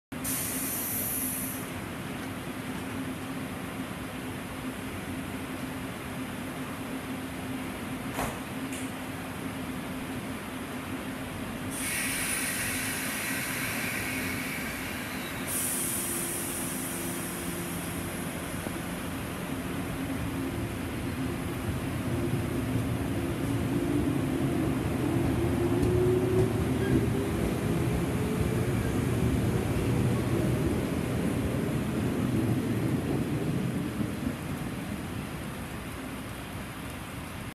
岡山電車区117系E-06編成サンライナー 笠岡駅発車
2017年9月27日撮影 サンライナーが発車するところです。
E-06編成
クハ117-19+モハ117-38+モハ116-38+クハ116-19